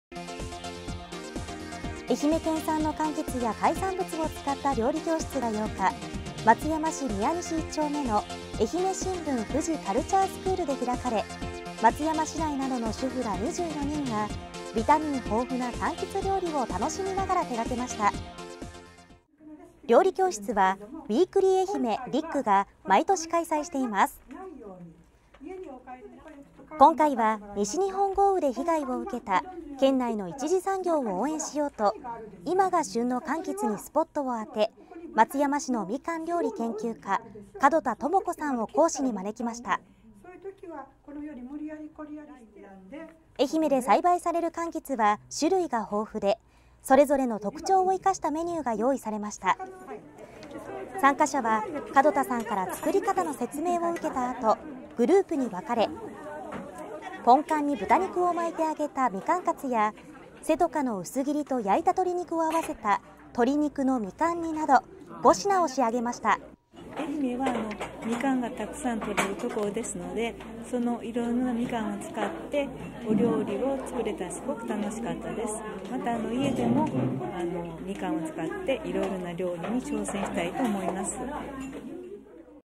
県産かんきつの美味凝縮 松山で料理教室・愛媛新聞
愛媛県産のかんきつや海産物などを使った料理教室が８日、松山市宮西１丁目の愛媛新聞・フジカルチャースクールであり、市内などの主婦ら２４人がなじみのないかんきつ料理を楽しみながら調理した。